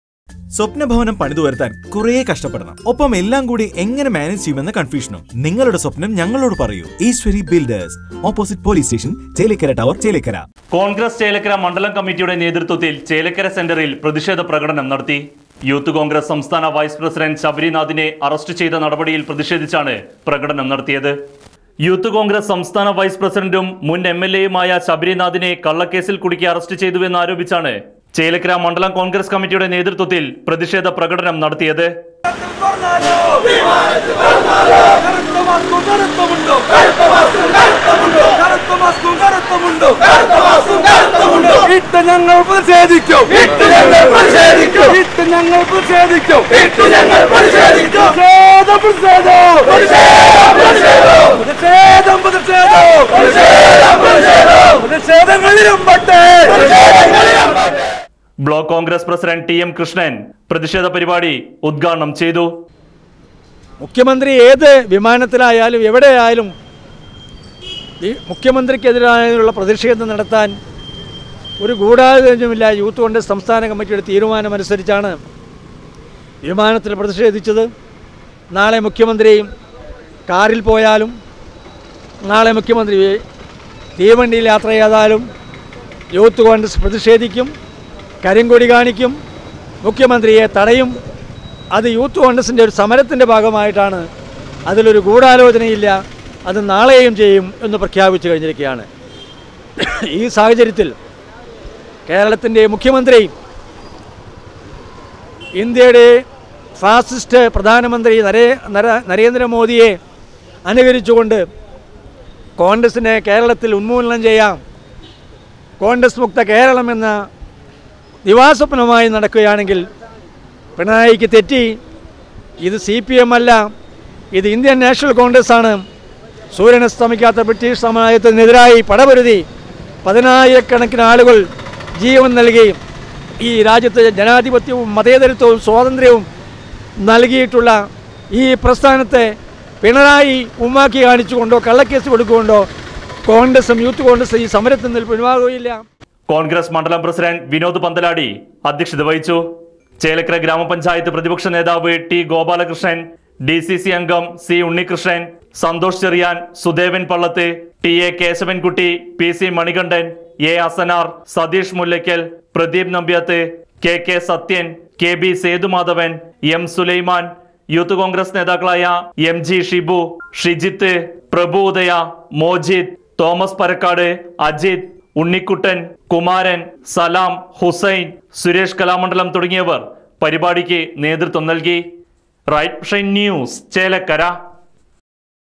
ചേലക്കര:: മുന് mla ശബരീനാഥിനെ അറസ്റ്റ് ചെയ്തതിൽ പ്രതിഷേധിച്ച് കോൺഗ്രസിന്റെ പ്രകടനം
ചേലക്കര:: യൂത്ത് കോൺഗ്രസ് സംസ്ഥാന വൈസ് പ്രസിഡന്റ് ശബരീനാഥിനെ അറസ്റ്റ് ചെയ്തതിൽ പ്രതിഷേധിച്ച് കോൺഗ്രസിന്റെ പ്രകടനം